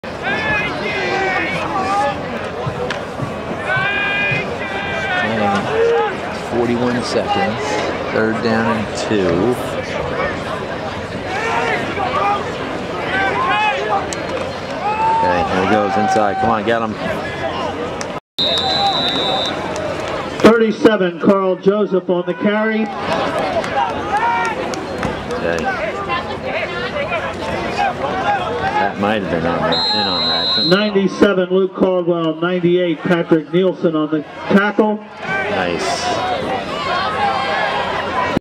Patrick Nilsen #98 Tackles running back in 2012 Shenandoah Game
From his left defensive end position Patrick Nilsen tackles Shenandoah running back